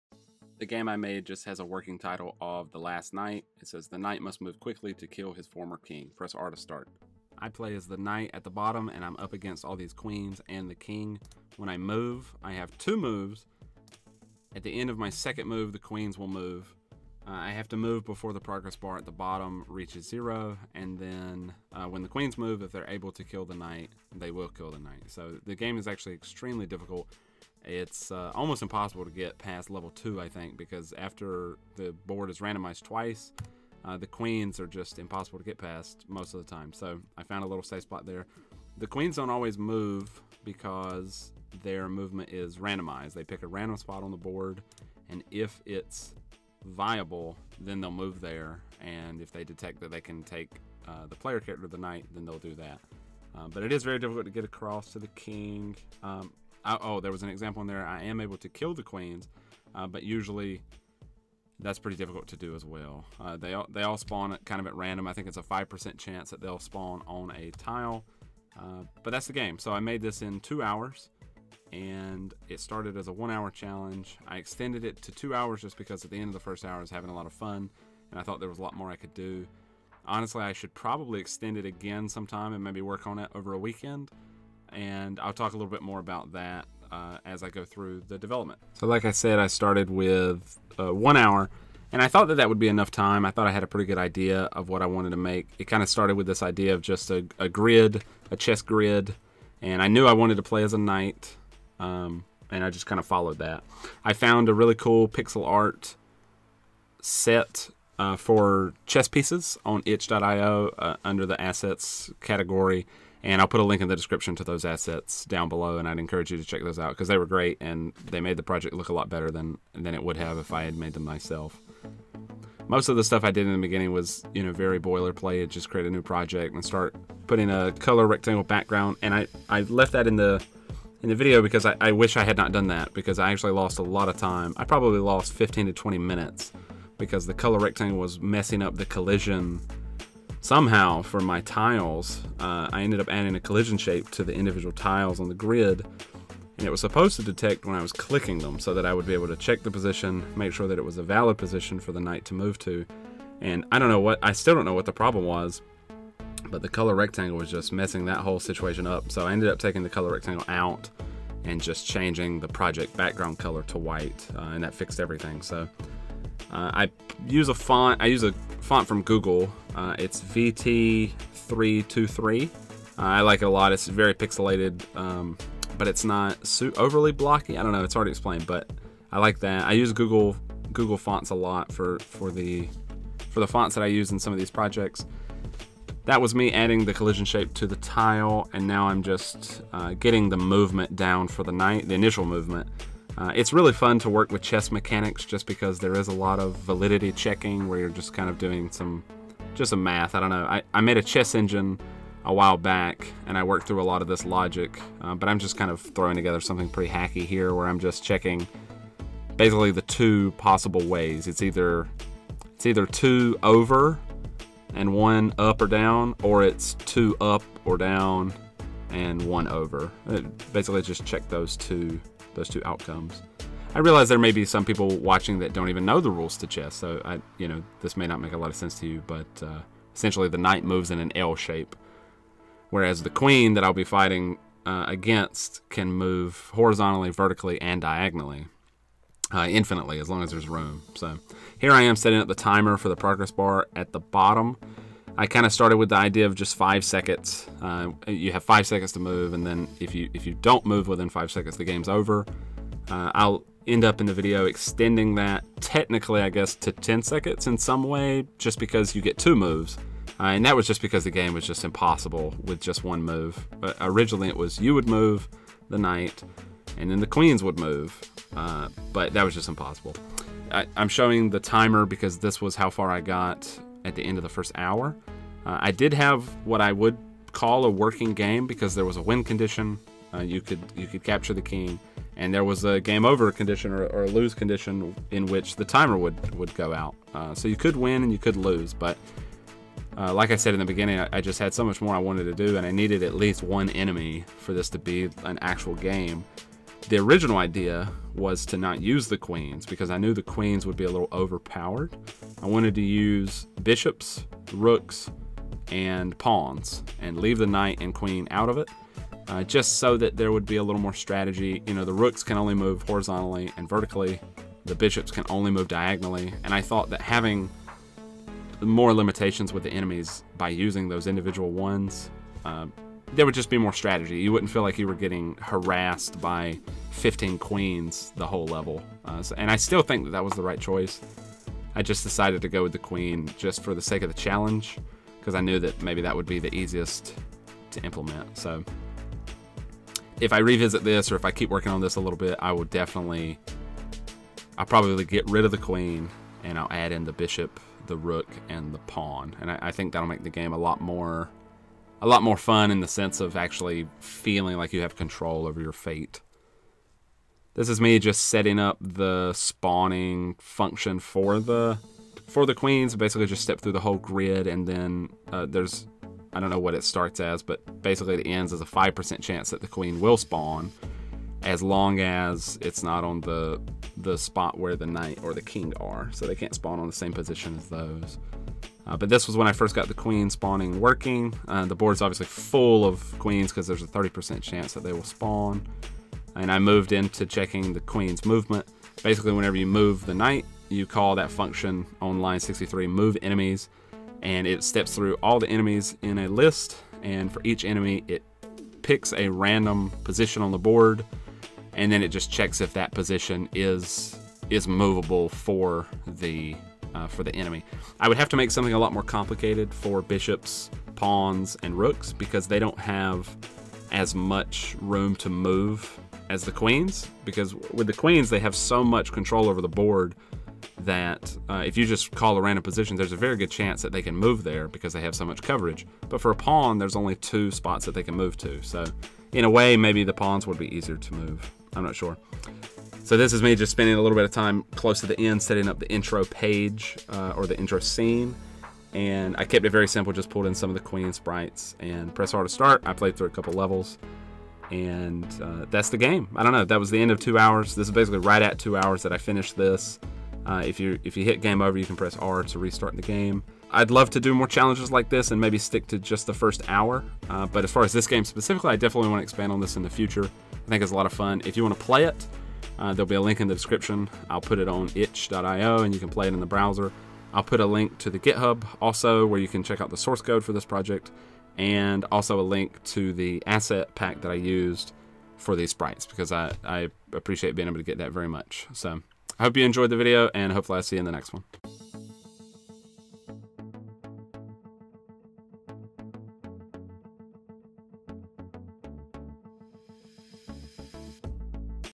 I Made an Impossible Chess Game in Godot in 2 Hours
I did a personal challenge and attempted to make a game in only 1 hour! It went pretty well but I did extend it to 2 hours and was able to finish something pretty fun by the end. I made a simple chess based game where you control the black knight and attempt to capture the king. In your way are randomly placed queens that will capture you if they can.

Check out the links below to play the game, check out the source code, and see the asset pack that I found and used.